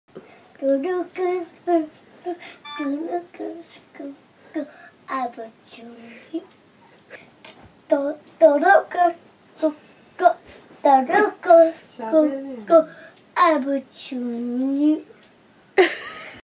海の楽しんごダンス！どどすこすこすこ
あのダンスは子供を引きつけるのかな〜。
楽しんごを初めてテレビでみたその晩のダンス！
あの決めの顔がたまりませんっ。

Kai dances wonderfully like a Japanese Comedian "Tanoshingo".
We love his face and pose at the end of the dance.